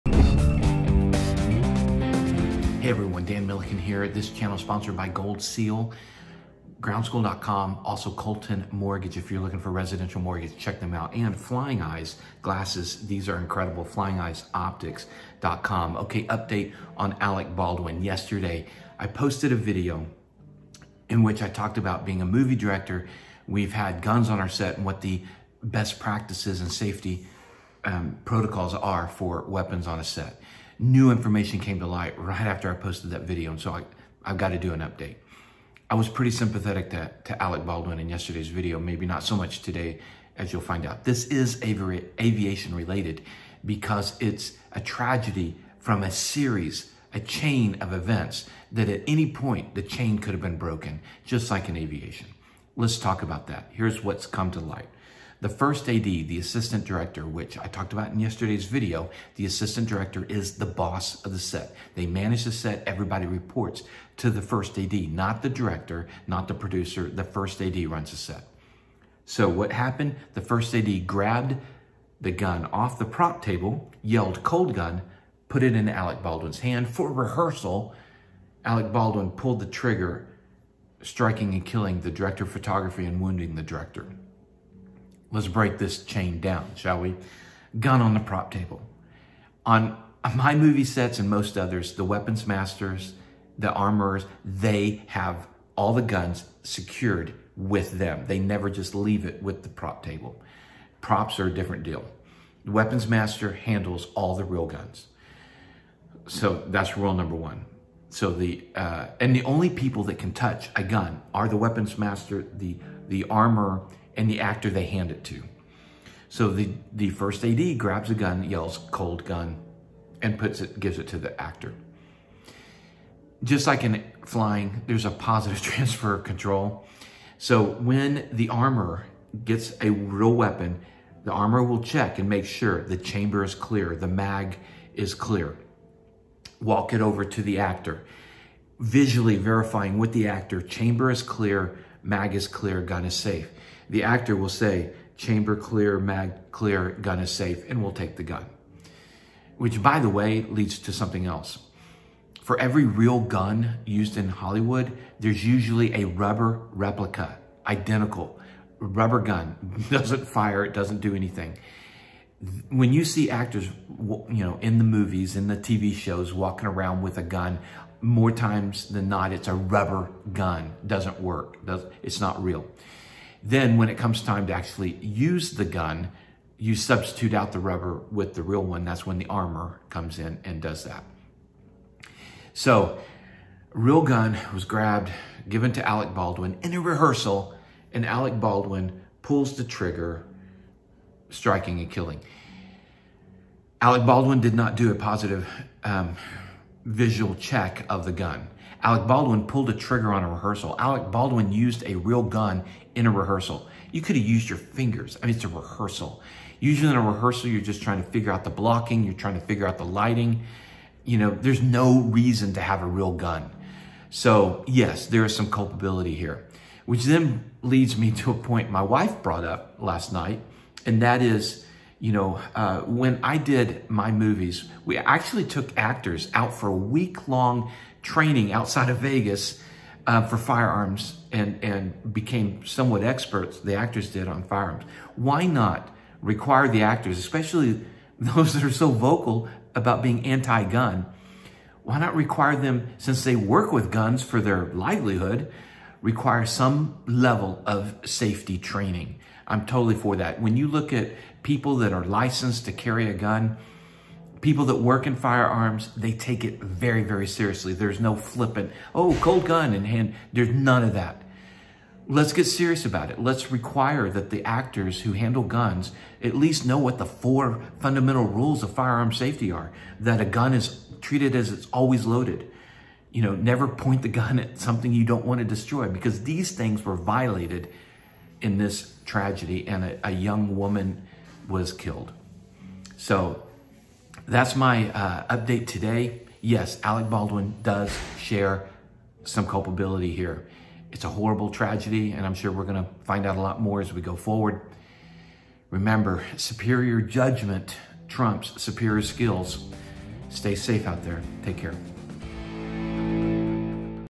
Alec Baldwin Tragedy Update from a Movie Director
New information came to light after I posted the explanation of how weapons work on a movie set.  That video from the day before is
This tragedy is similar to aviation accidents int hat it's a series of events in which had proper safety protocols and industry best practices been followed, tragedy would have been averted.